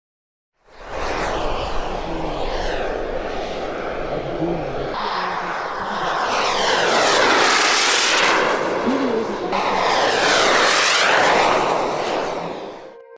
Wow! This is an Amazing RC Blackbird SR-71 Model with Turbine
Pilot: Matthias Blaß
Builder: Walter Blaß
Model: Lockheed SR-71 Blackbird
Engine: 2x Frank Turbine
Thrust: 32Kg
Tank: 10 Liter Cerosene
Wingspan: 2.40m
Length: 4.10m
Take-off weight: ~36Kg
Scale: 1:7.5

Event: RC Airlinertreffen in Oppingen  2022